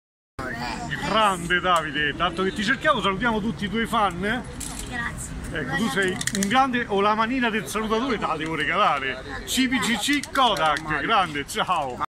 Ciribiribi Kodak Davide Marotta saluta i fan col Salutatore
Pinocchio - Film di Matteo Garrone: Il Grillo parlante è Davide Marotta il Notissimo ciribiribi Kodak che saluta i fan col Salutatore.
➜ FACEBOOK IL SALUTATORE:
➜ PAGINA